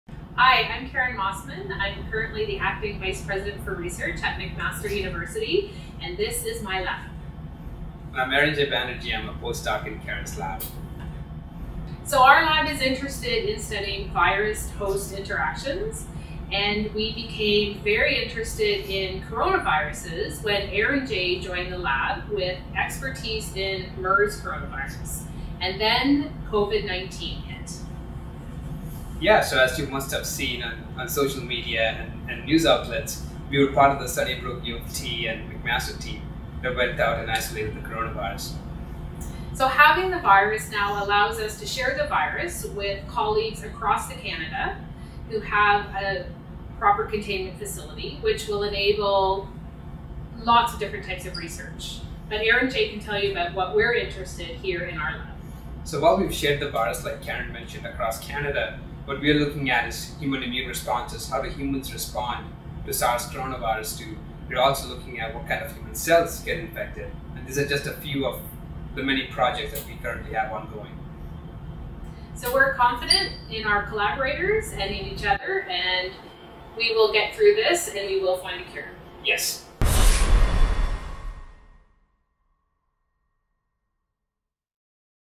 Virus-host interactions
Karen Mossman and Arinjay Banerjee of McMaster University were part of the Sunnybrook Health Sciences Centre, University of Toronto and McMaster teams that isolated SARS-CoV-2 the agent responsible for the ongoing outbreak of COVID-19. They are currently leading studies in virus-host responses.

Video Transcription:

Karen:

Hi. I’m Karen Mossman. I’m currently the acting vice-president for research at McMaster University, and this is my lab.

Arinjay:
I’m Arinjay Banerjee. I’m a post-doc in Karen’s lab.

Karen:
Our lab is interested in studying virus-host interactions, and we became very interested in coronaviruses when Arinjay joined the lab with expertise in MERS coronavirus, and then COVID-19 hit.

Arinjay:
Yeah, so as you must have seen on social media and news outlets, we were part of the Sunnybrook, U of T and McMaster team that went down and isolated the coronavirus.

Karen:
Having the virus now allows us to share the virus with colleagues across Canada who have a proper containment facility, which will enable lots of different types of research, but Arinjay can tell you about what we’re interested in here in our lab.

Arinjay:
While we’ve shared the virus, like Karen mentioned, across Canada, what we’re looking at is human immune responses, how the humans respond to SARS coronavirus-2. We’re also looking at what kind of human cells get infected, and this is just a few of the many projects that we currently have ongoing.

Karen:
We’re confident in our collaborators and in each other, and we will get through this, and we will find a cure.

Arinjay:
Yes.